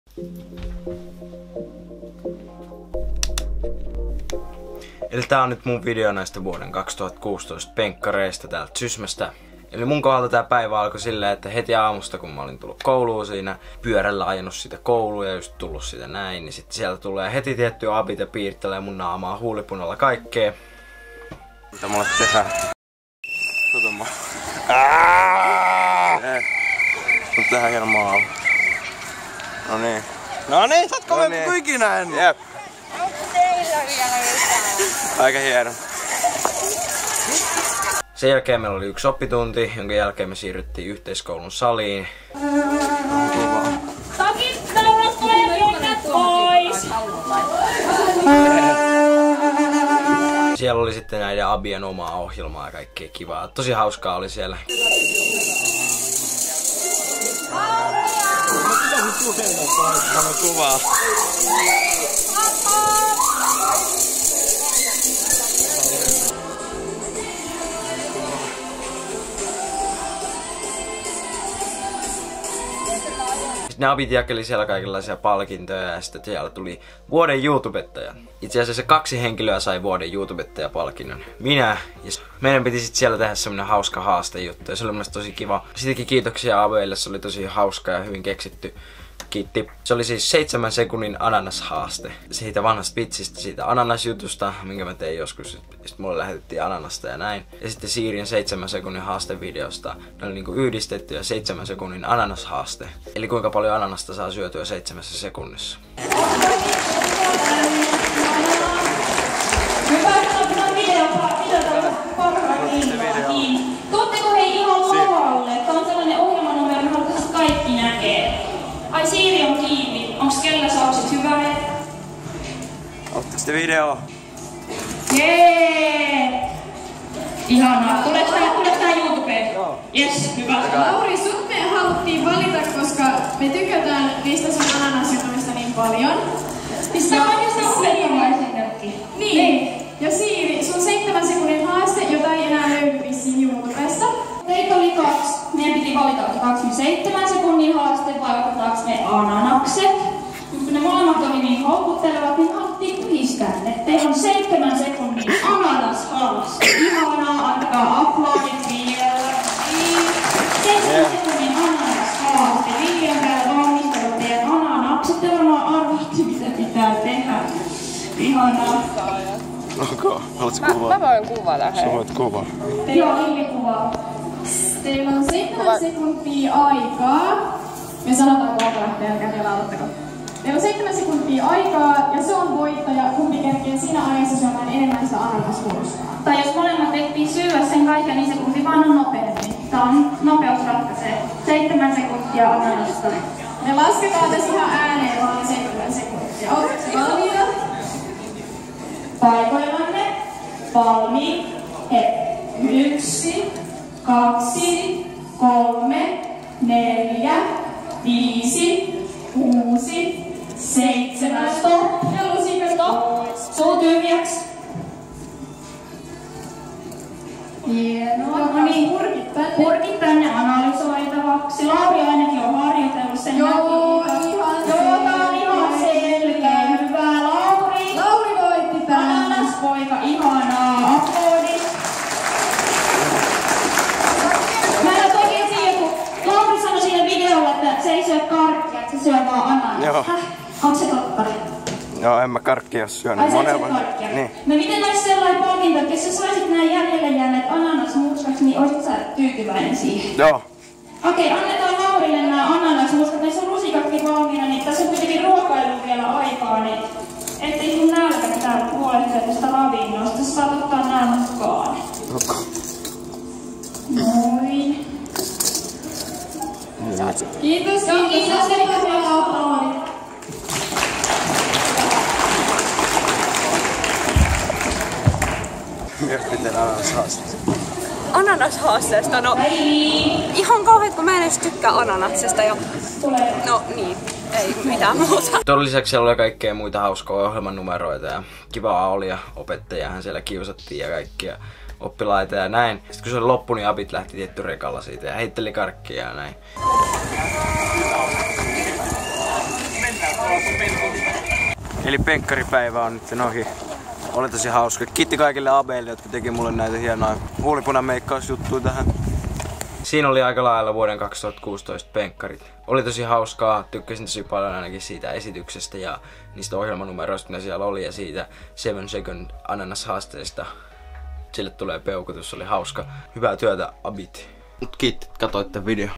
Penkkarit 2016
Tänään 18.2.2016 oli siis penkkarit eli penkinpainajaiset täällä Sysmässä ja abit tietysti keksivät kaikkea hauskaa tapahtumaa päivälle.
HAUSKAA OLI, KIITOS ABEILLE

Hei, olen Lauri "LATE" Toivonen ja teen YouTubeen videoita.